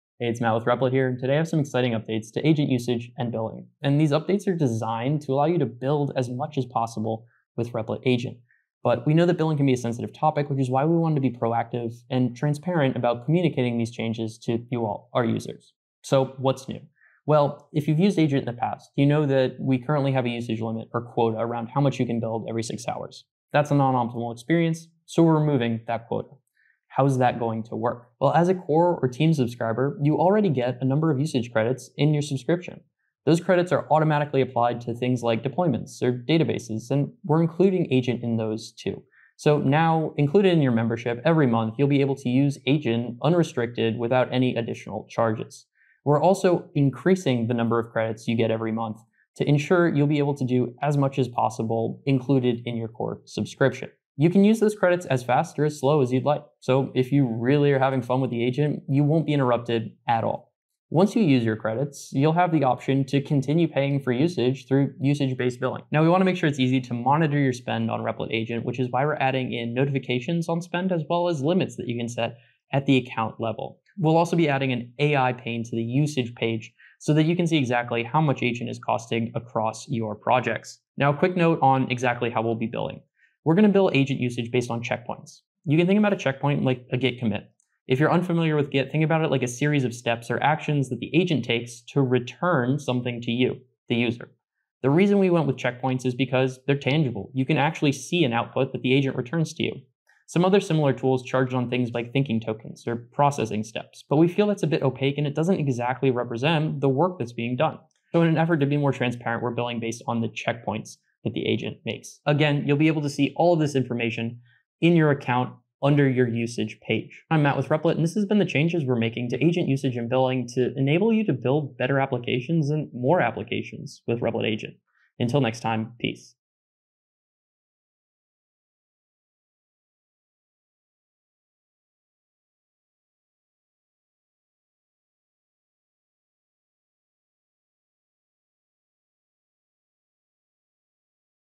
Agent Usage & Billing Updates
Updates to Replit Agent: New billing system makes building AI-powered apps easier and more transparent.

Key Timestamps:
00:00 - Introduction to Agent billing changes
00:28 - Core/Team subscriber credit system explained
01:05 - New monitoring features and spend controls
01:20 - Checkpoint-based billing system overview
01:57 - Wrap-up and final thoughts

Important Updates:
• Moving from 6-hour quotas to credit-based system
• New usage monitoring and spend limit features
• Transparent checkpoint-based billing model

💭 What features would you like to build with Replit Agent